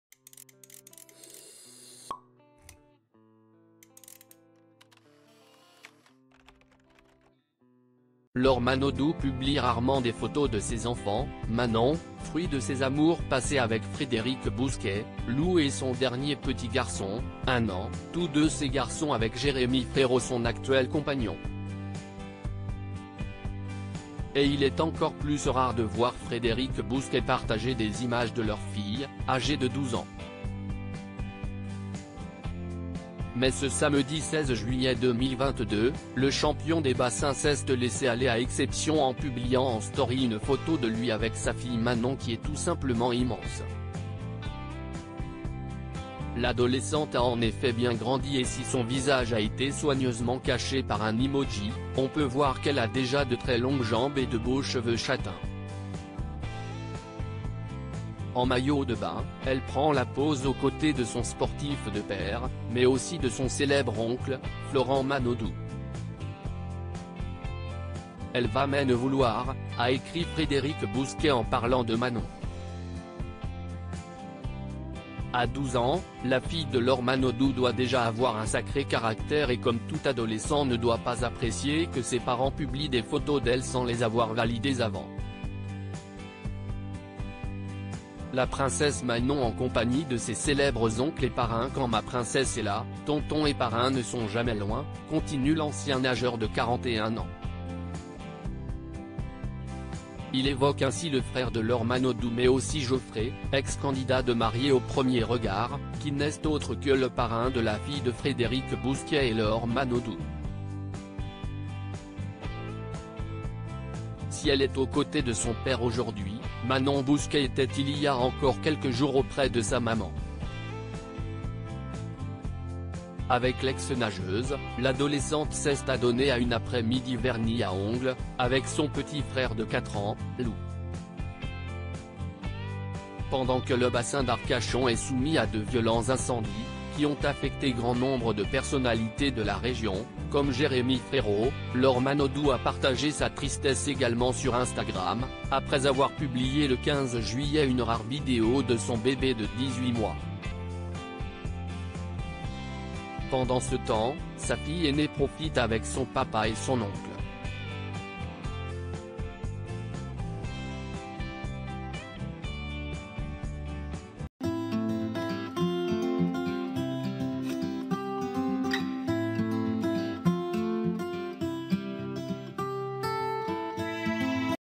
Frédérick Bousquet : L'ex de Laure Manaudou publie une belle photo de leur fille Manon, immense à 12
Frédérick Bousquet : L'ex de Laure Manaudou publie une belle photo de leur fille Manon, immense à 12 ans !